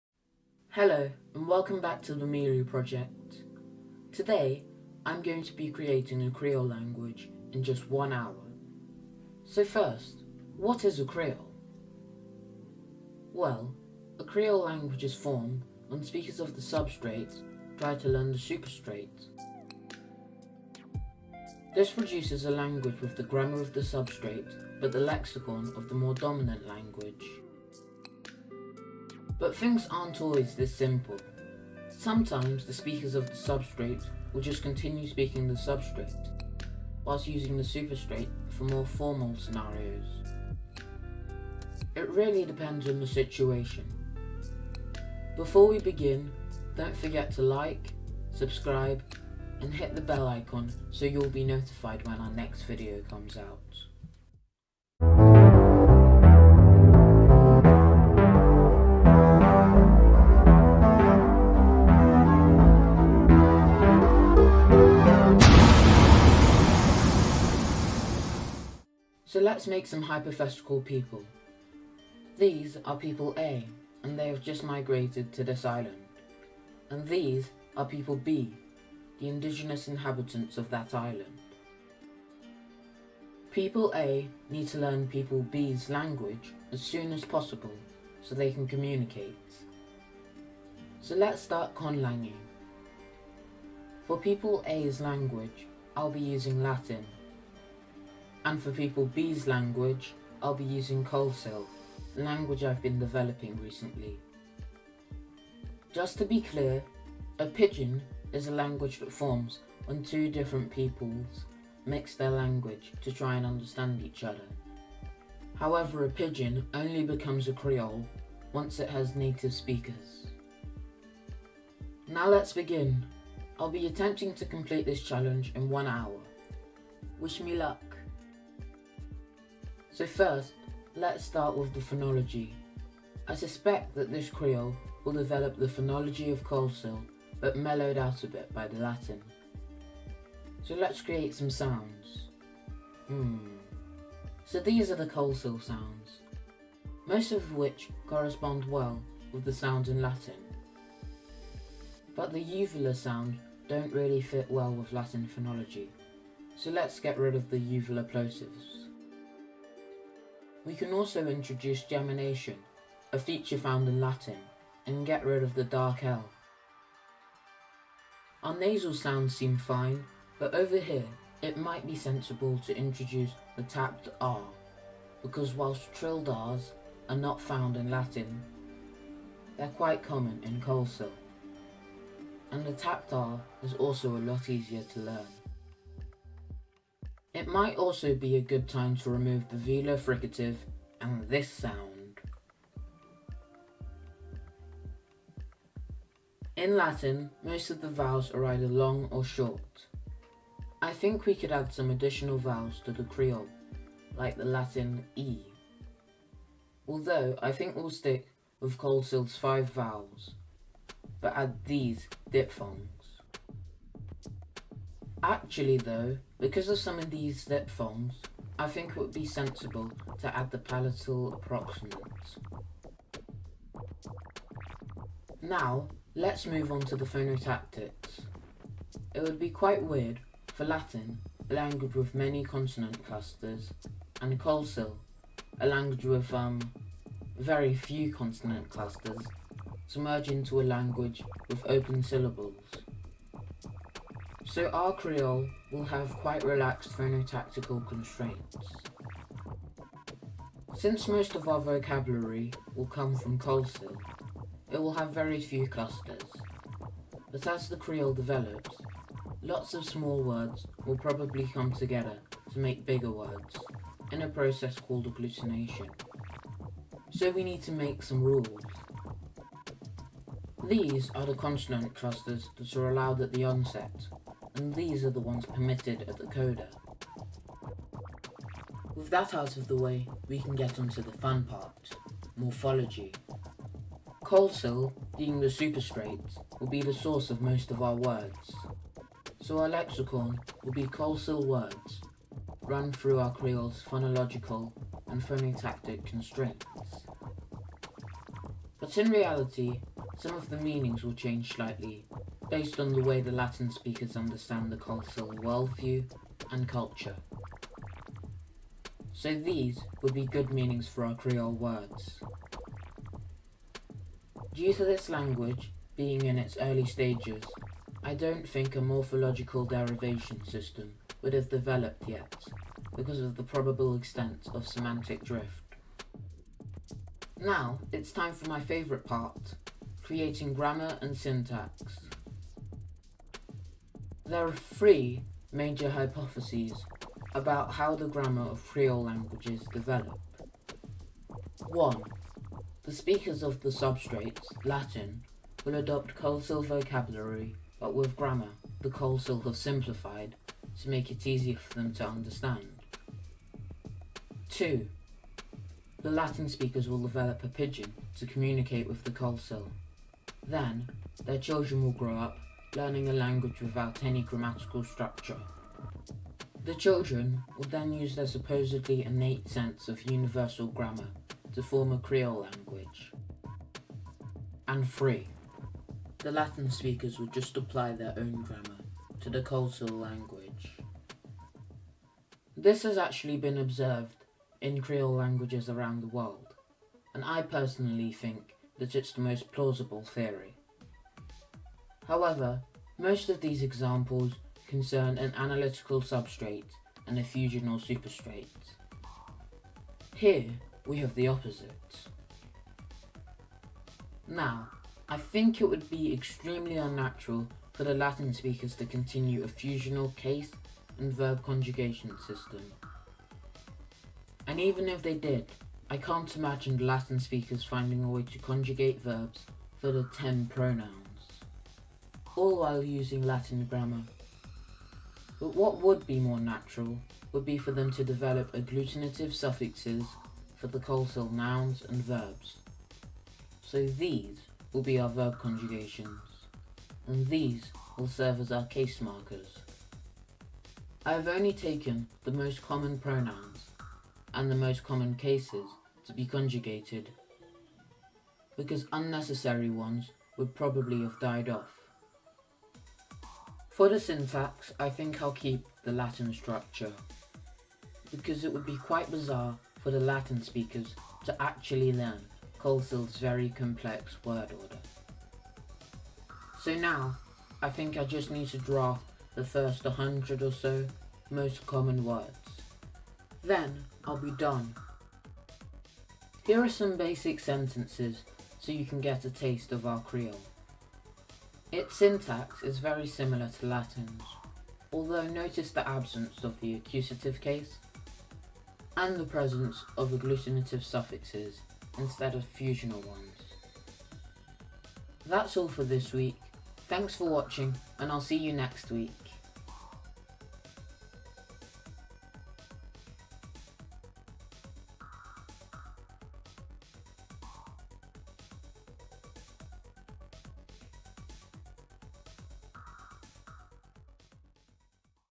Conlang Speedrunning | Kolsil Creole
Creole languages are formed when two languages mix, today I will making a Kolsil-Latin creole in 60 minutes!